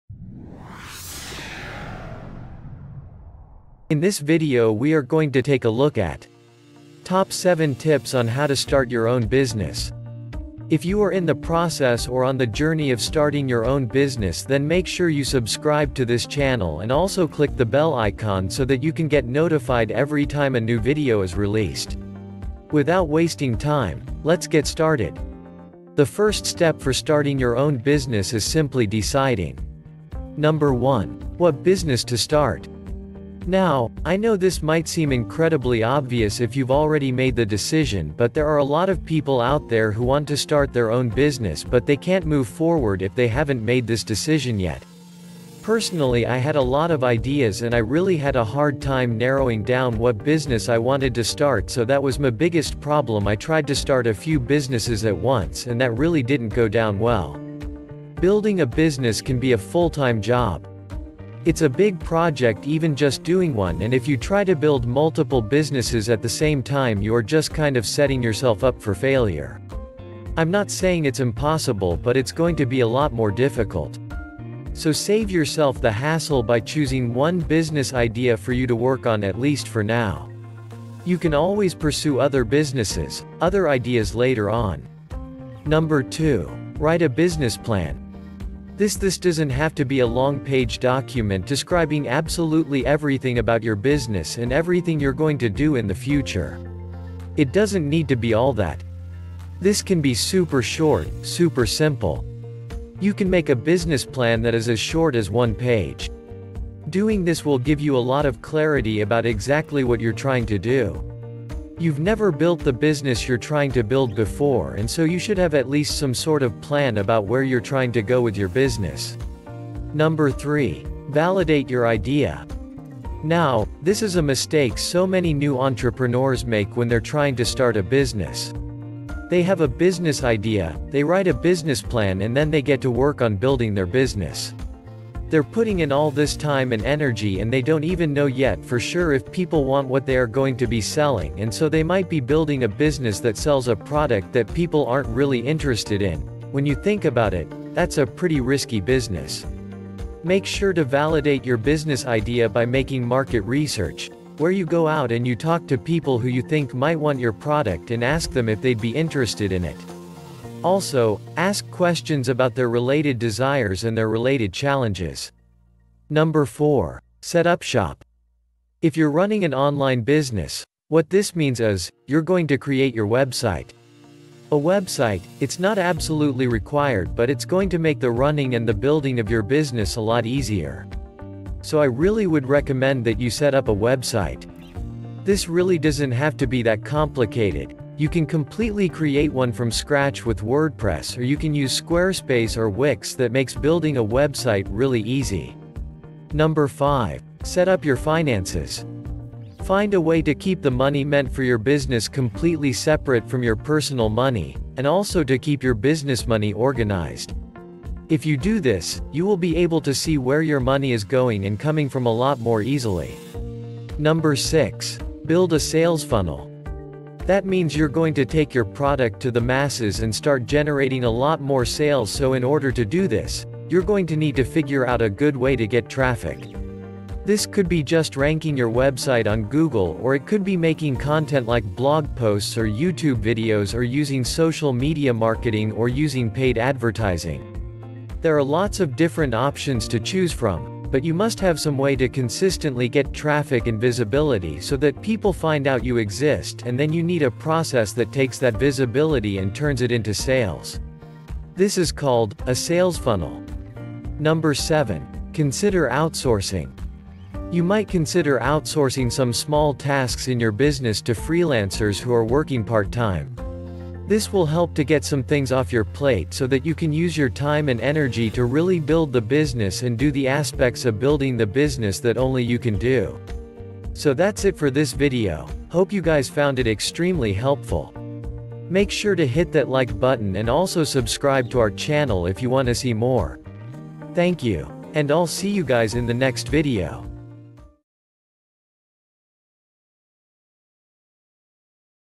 TOP 7 TIPS TO START YOUR BUSINESS (SMALL BUSINESSES OWNERS)
A breakdown of 7 of the most important business tips to start your business.

If you're planning to become a small business owner, then this video is for you.

Timecodes
0:00
0:09 - how to start a business
0:31 - what business to start
1:27 - write a business plan
2:02 - validate your ideas
2:47 - set up shop
3:18 - set up your finances
3:35 - build a sales funnel
4:16 - consider outsourcing